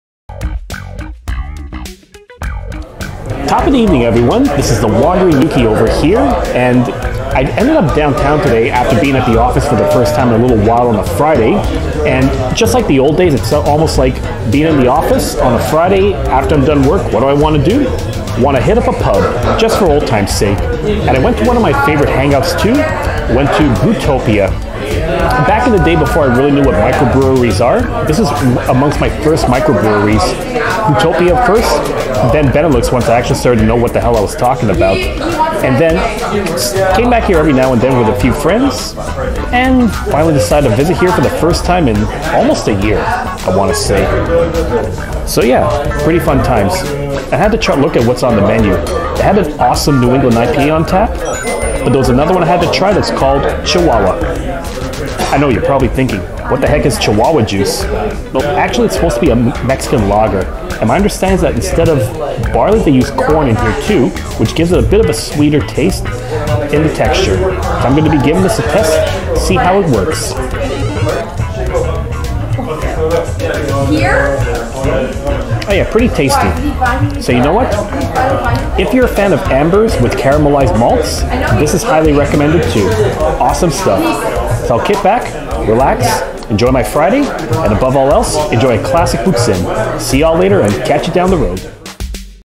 Brutopia - Chihuahua Mexican Lager
Friday evening in Montreal was a perfect excuse to stick around for a pint or two. Decided to revisit a favourite spot for the first time in close to a year.

One of my first microbrewery experiences before really getting to know microbrweries, Brutopia is still a great place to grab a drink. Ended up trying out their Mexican styled lager just because of the name (who wouldn't want to grab a pint of Chihuahua extract?)